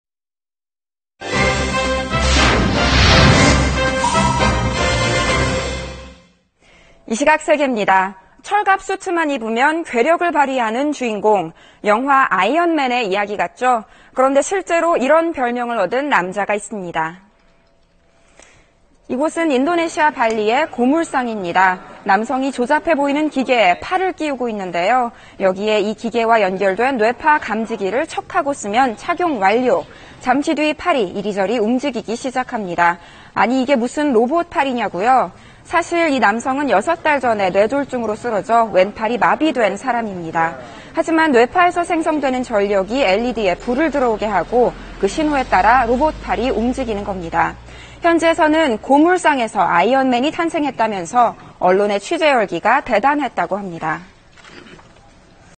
'아이언맨' 별명 얻은 '뇌졸중 남성'…사연은? / SBS
철갑 슈트만 입으면 괴력을 발휘하는 주인공, 영화 아이언맨의 이야기 같죠?

그런데 실제로 이런 별명을 얻은 남자가 있습니다.

이곳은 인도네시아 발리의 고물상입니다.